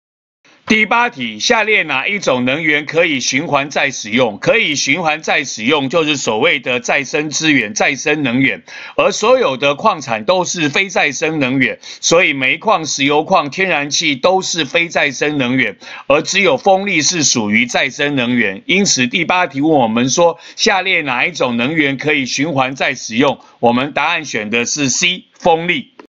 106學年上9年級理化三段第08題
問哪一種能源可循環再利用。